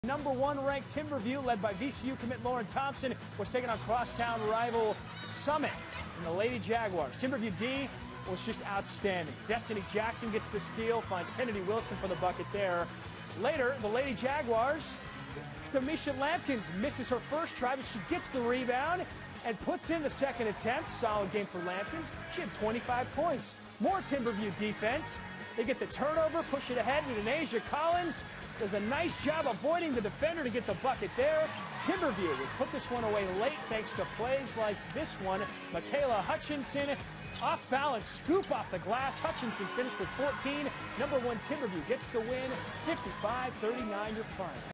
Week 3 - Girls Hoops - Mansfield Timberview Lady Wolves at Mansfield Summit Lady Jaguars
Timberview proves why they are number 1 in the state with the win over Summit.